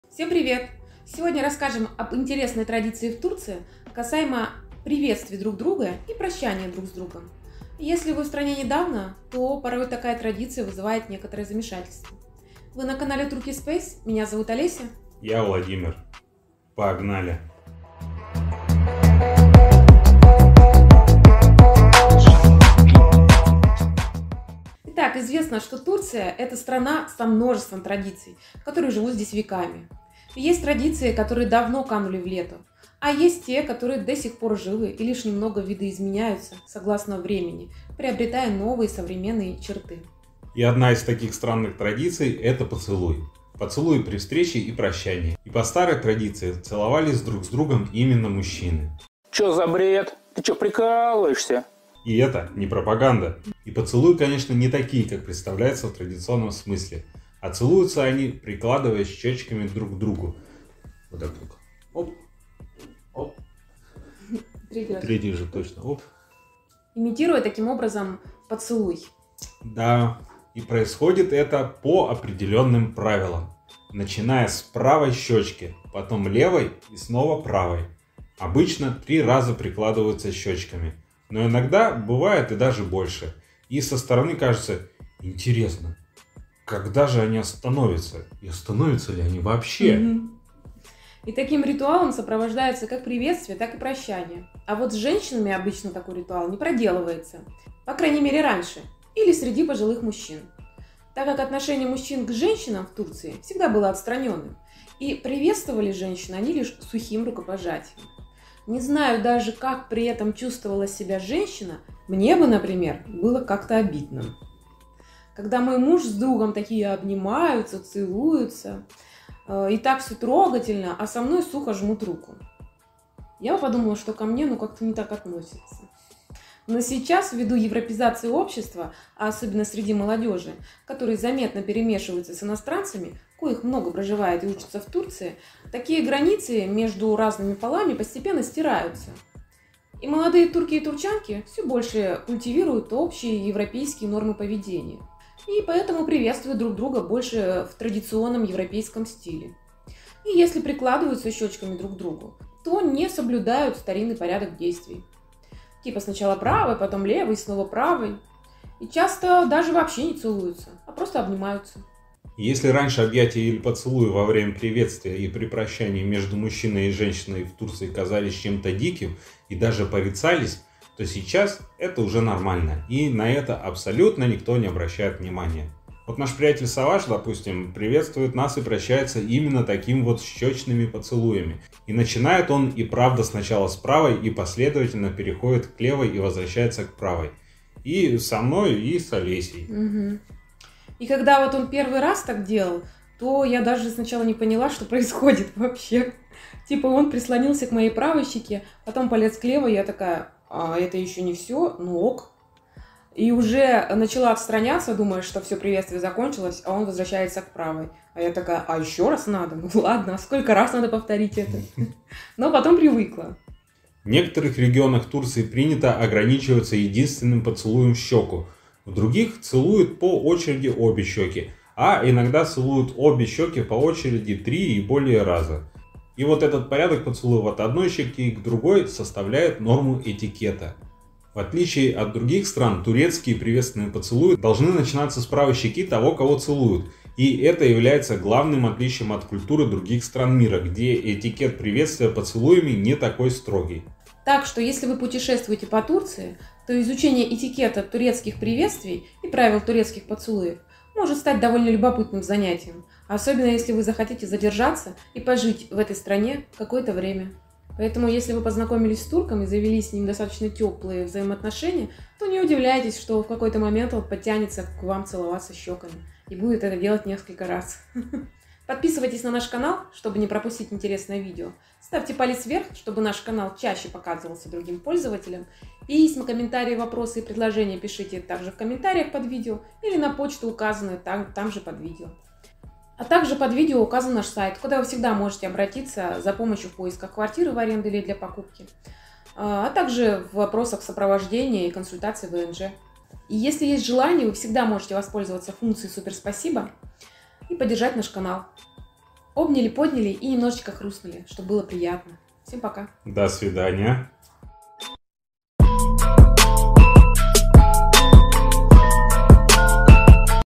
Что должны знать ВСЕ о приветствиях в Турции
Особенности этикета приветствий и прощаний в Турции и турецкой культуре. Как древние традиции претерпели изменения и смогли пережить веяния нового времени. К чему быть готовыми при взаимоотношении с турками.

✅ Сайт агенства недвижимости, где мы помогаем найти квартиру под ВНЖ и оказывают помощь в оформлении ВНЖ: